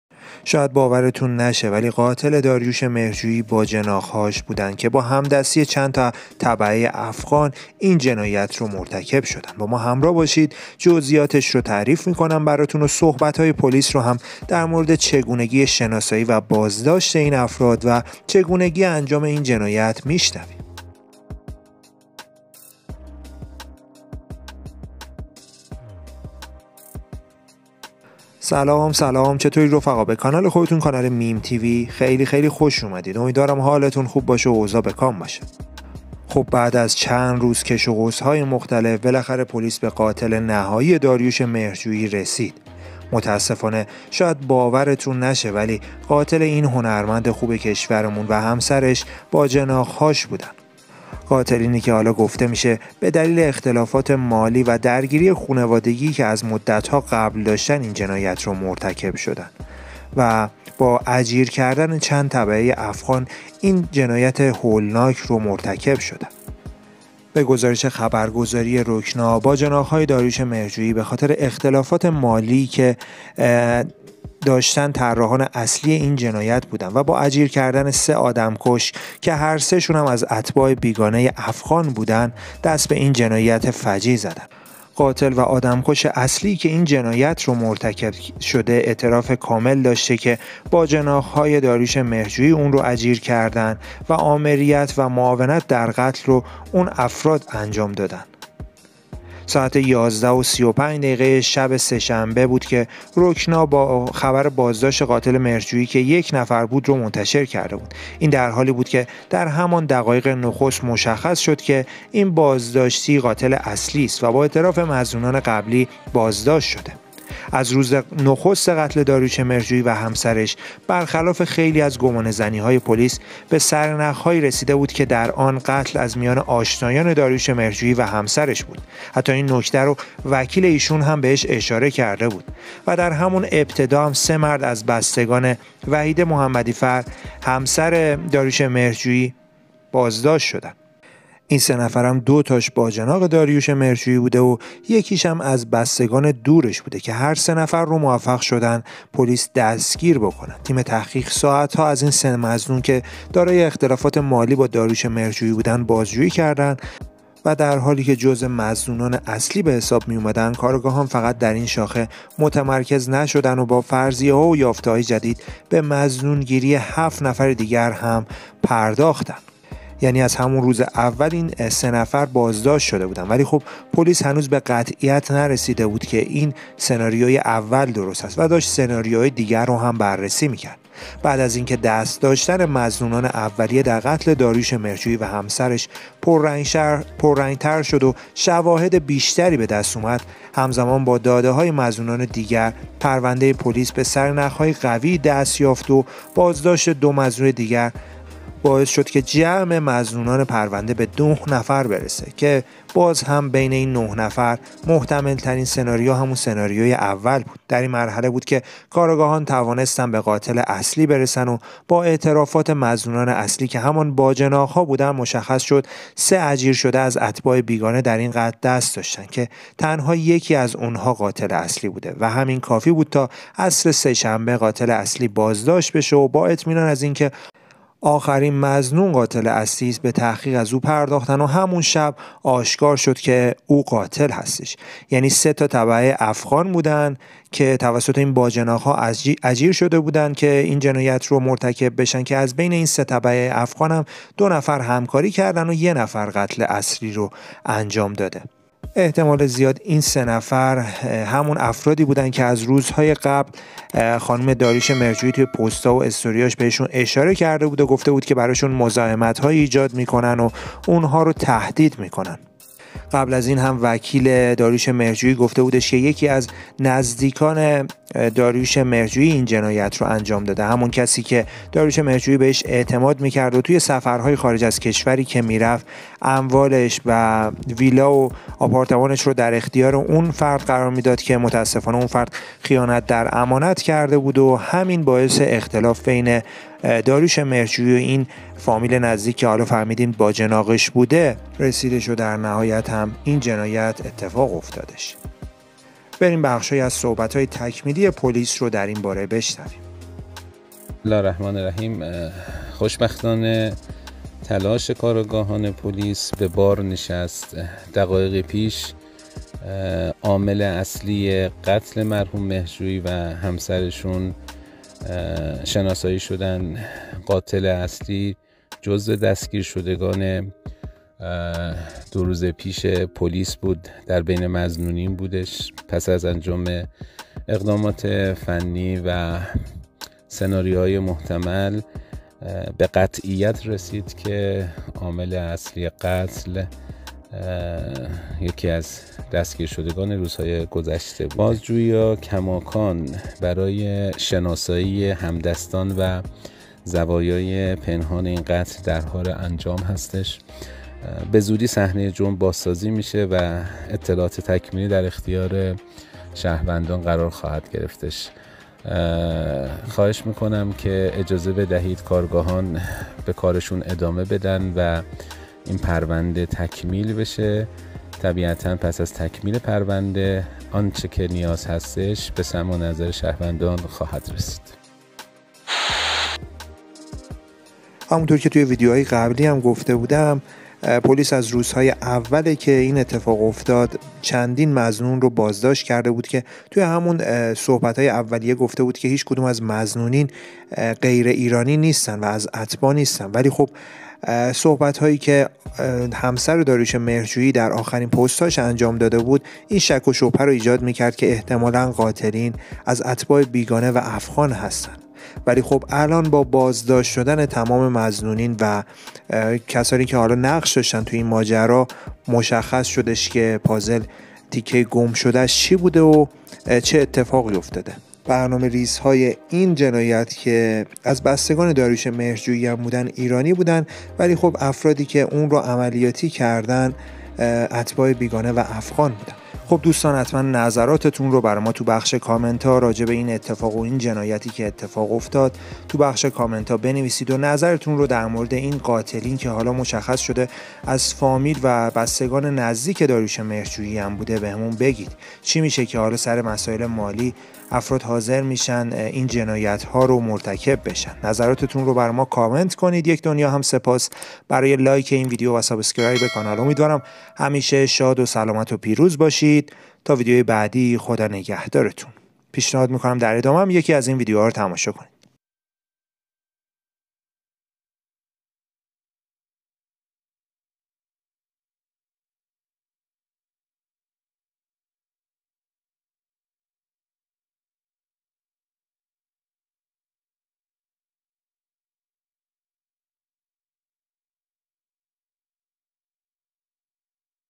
قات.ل اصلی مهرجویی و همسرش مشخص شد | دو باجناق با همکاری سه تبعه افغان + علت جنا.یت و اعترافات
باجناق_مهرجویی
بعد از اینکه دست داشتن مظنونان اولیه در قتل داریوش مهرجویی و همسرش پررنگ تر شد همزمان با داده های مظنونان دیگر پرونده پلیس به سرنخ هایی قوی دست یافت و با بازداشت ۲ مظنون دیگر تعداد مظنونان به ۹ نفر رسید

قات.ل اصلی مهرجوییو همسرش مشخص شد | دو باجناق با همکاری سه تبعه افغان + علت جنا.یت و اعترافات

آخرین وضعیت پرونده مهرجویی و دستگیری مظنونین جدید با ما همراه باشید اخرین وضعیت رو بهتون میگم صحبت های پلیس رو می شنویم درباره زمان خاکسپاری مهرجویی هم صحبت می کنیم
سلام

 روز نخست قتل داریوش مهرجویی و همسرش برخلاف خیلی از گمانه زنی ها پلیس به سرنخ هایی رسید که در آن قتل از میان آشنایان داریوش مهرجویی و همسرش بوده است و در همان ابتدا ۳ مرد از بستگان وحیده محمدی فر - ۲ باجناق و یکی از بستگان دورشان - بازداشت شدند و تیم تحقیق ساعت ها از این ۳ مظنون که دارای اختلافات مالی با داریوش مهرجویی بودند بازجویی کردند و در حالی که جزو مظنونان اصلی به حساب می آمدند کارآگاهان فقط در این شاخه متمرکز نشدند و با فرضیه ها و یافته های جدید به مظنون گیری ۷ نفر دیگر پرداختند .

در این مرحله بود که کارآگاهان توانستند به قاتل اصلی برسند و با اعترافات مظنونان اصلی که همان باجناق ها بودند مشخص شد ۳ اجیر شده از اتباع بیگانه در این قتل دست داشته اند که تنها یکی از آنها قاتل اصلی بوده است و همین کافی بود تا عصر سه شنبه قاتل اصلی بازداشت شود و با اطمینان از اینکه آخرین مظنون قاتل اصلی است به تحقیق از او پرداختند و همان شب آشکار شد که او قاتل است.

همان شب رکنا خبر بازداشت قاتل اصلی را منتشر کرد حالا بعد از گذشت ۲ روز دفتر سخنگوی ناجا با تایید خبر بازداشت قاتل اصلی داریوش مهرجویی و همسرش صراحتا اعلام کرده است که قاتل دو روز پیش یعنی همان روز انتشار خبرش در رکنا بازداشت شده است